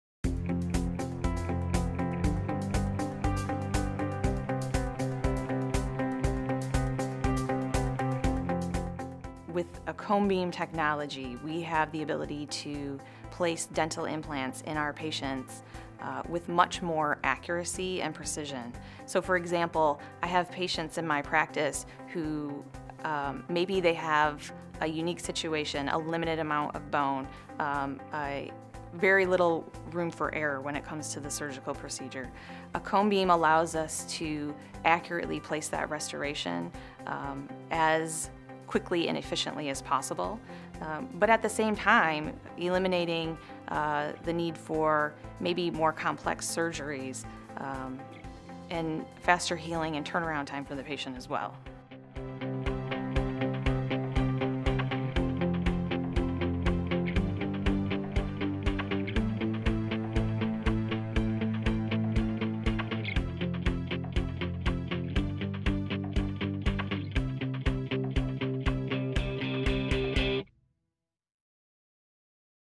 Efficient Dental Surgery Leads to Faster Healing
Dr. Sheena Allen explains how efficient surgery leads to faster healing times due to the advancements of digital dentistry.

To learn more visit GoToAPro.org.